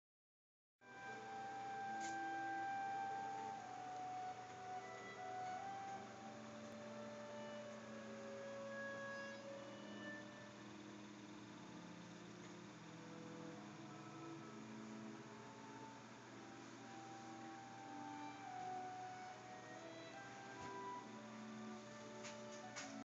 WRS V sleeping again.3gp
REM sleep, listening to classical music